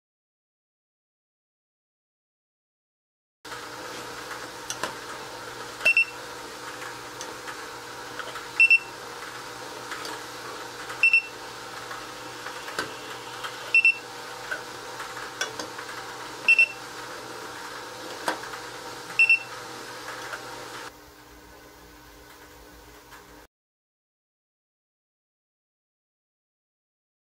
SNEED-JET Titan T6 Automatic Inkjet Coder Printing Down on Tin Jar Lids
You're watching the SNEED-JET Titan T6 Automatic Inkjet Coder Printing Down on Tin Jar Lids.

The SNEED-JET Titan T6 Automatic Inkjet Printer prints static and variable information such as date codes, lot codes, batch codes, counters,, barcodes and much more. Print with black, white, yellow, blue, red, green and invisible inks on a variety of substrates such as glass, plastic, metal, aluminum, concrete, boxes, etc. Change ink colors in less than 30 seconds!

833-9-CODING